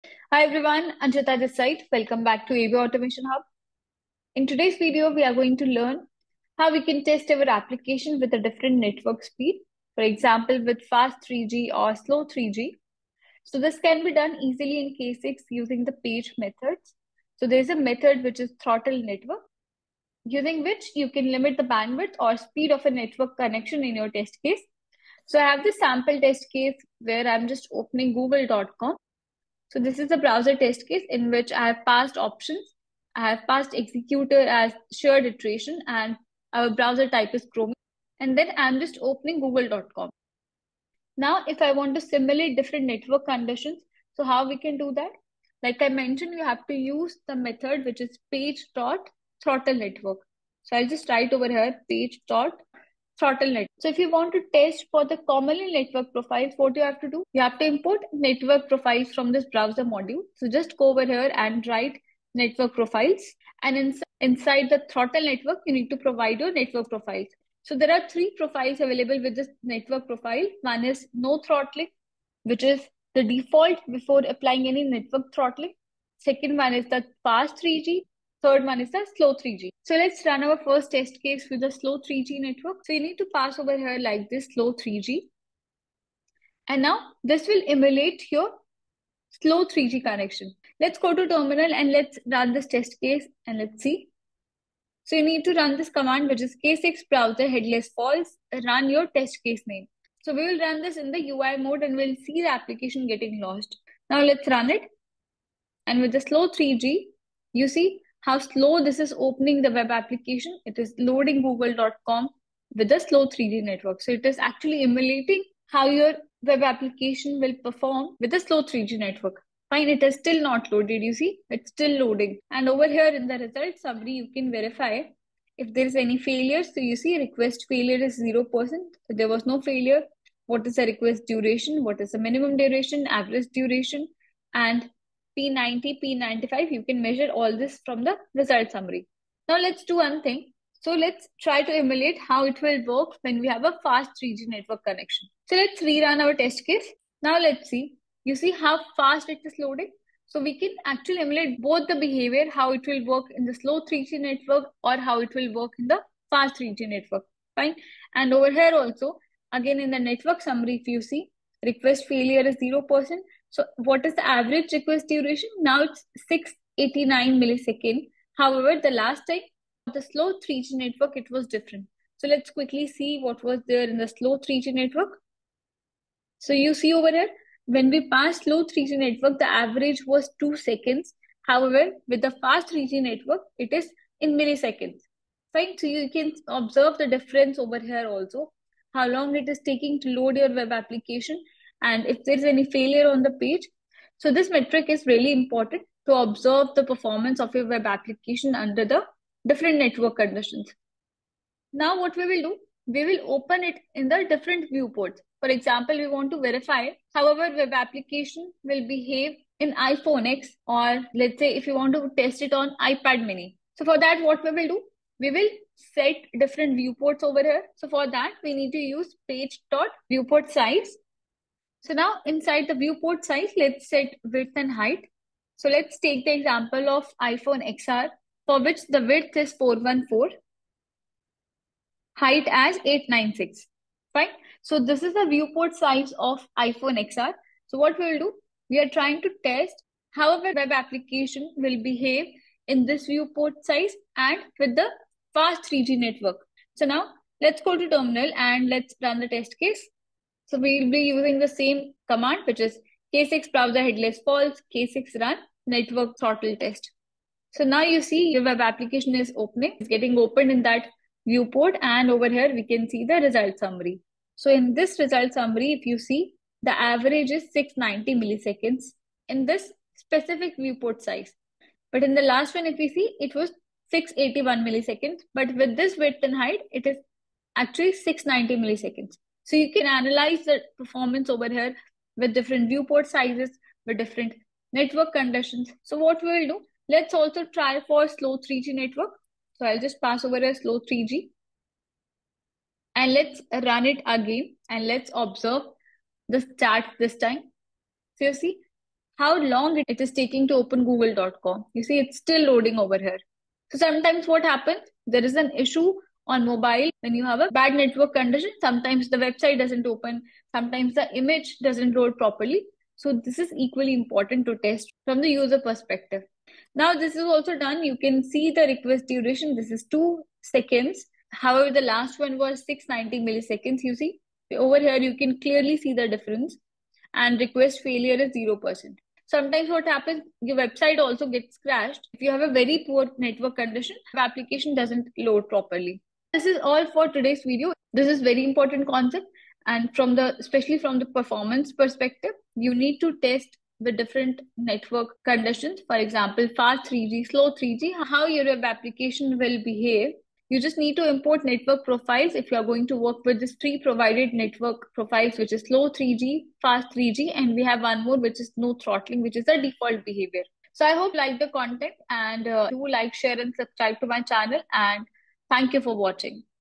k6 || NetworkThrottle || How to emulate different network conditions like Slow 3G and Fast 3G
In this video, I have explained how to perform Network Throttling in k6 for different network conditions like Fast 3G and Slow 3G.